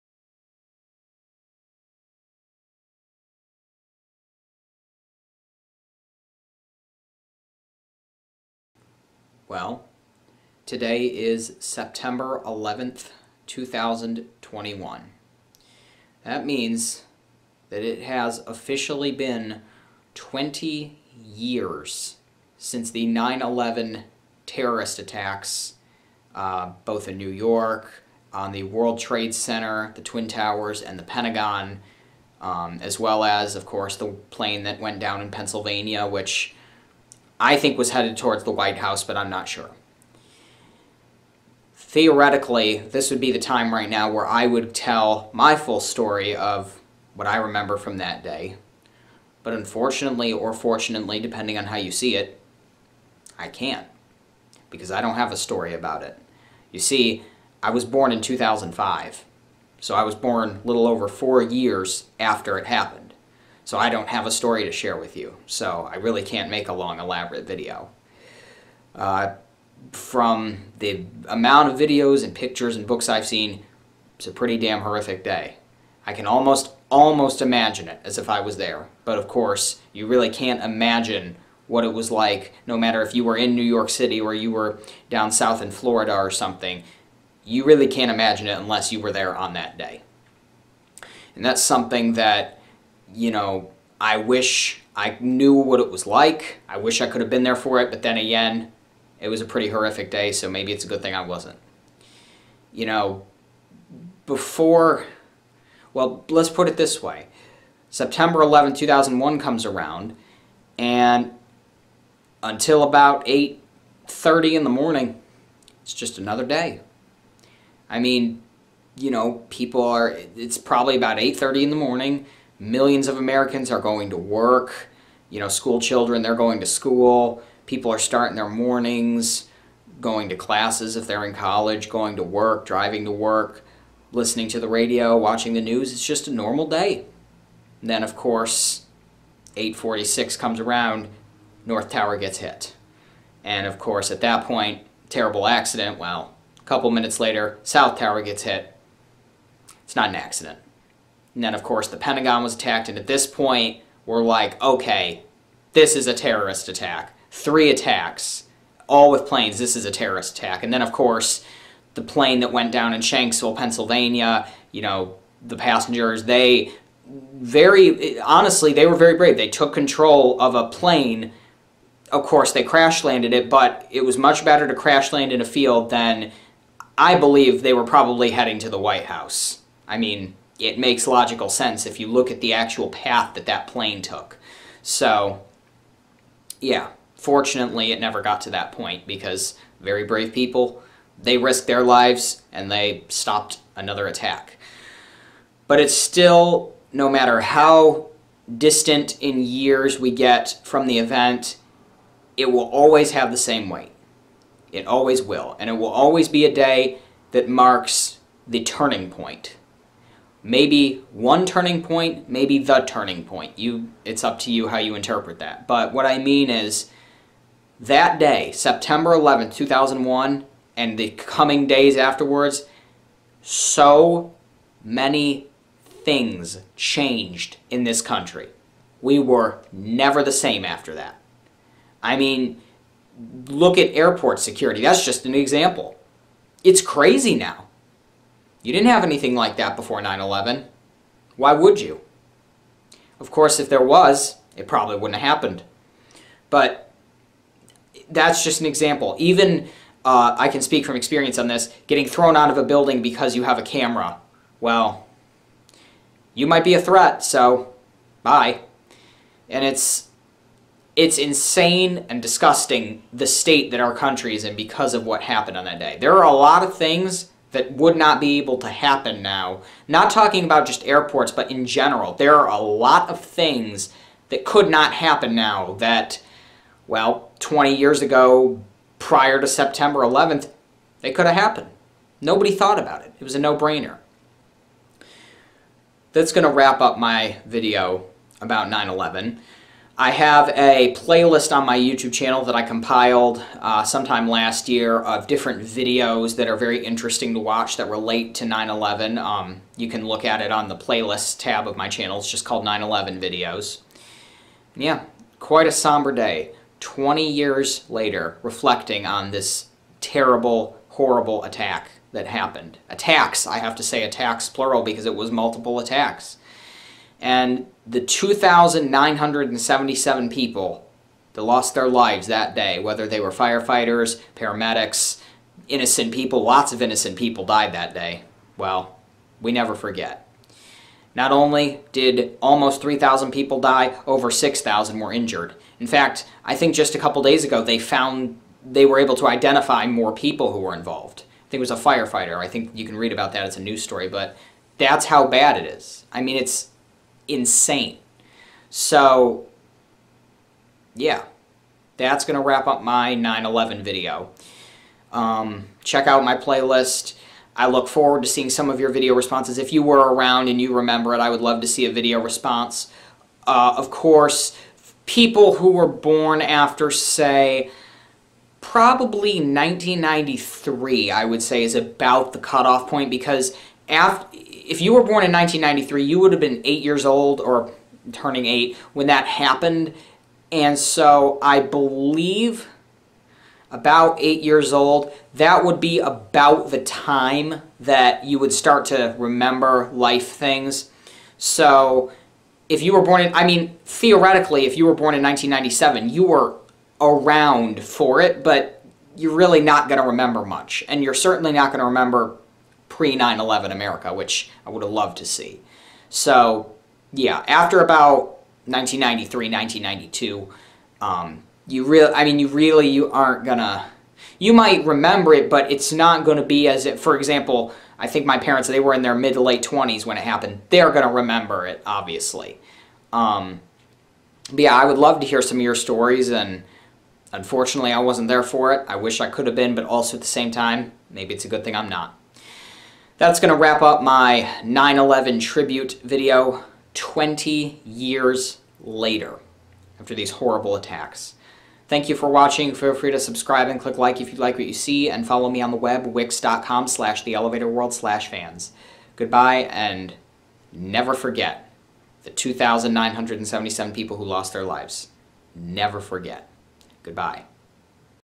9/11 Attacks: On The 20th Anniversary
Today, September 11, 2021, is the 20th anniversary of the tragic 9/11 terrorist attacks. Here is my tribute video.

Posted at 8:46 AM, the same minute that American Airlines Flight 11 hit the North Tower of the World Trade Center.